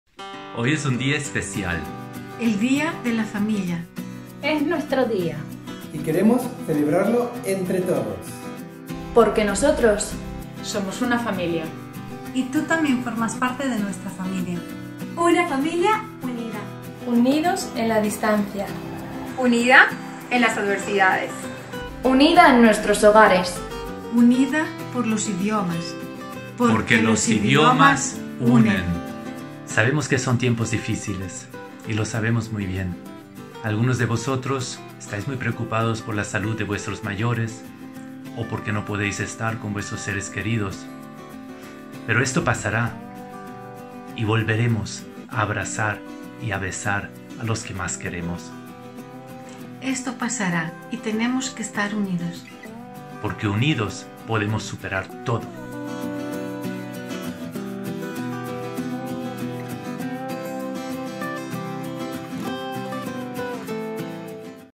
Feliz Día de la Familia 🥰👨‍👩‍👧‍👦 A Casa das Linguas
Hoy es un día muy especial, ¡EL DÍA INTERNACIONAL DE LA FAMILIA! 😍

Desde A Casa das Linguas sabemos que estamos viviendo días muy difíciles, muchos de nosotros estamos separados de nuestras familias y seres queridos. Y por eso, pensamos que ahora más que nunca, debemos recordar este día tan especial, que seguimos unidos en la distancia, y que esto PRONTO PASARÁ.

Nosotros somos una familia unida, y vosotros también formáis parte de esta familia que forma A Casa das Linguas.

¡GRACIAS A TODOS POR SEGUIR SIÉNDOLO! ❤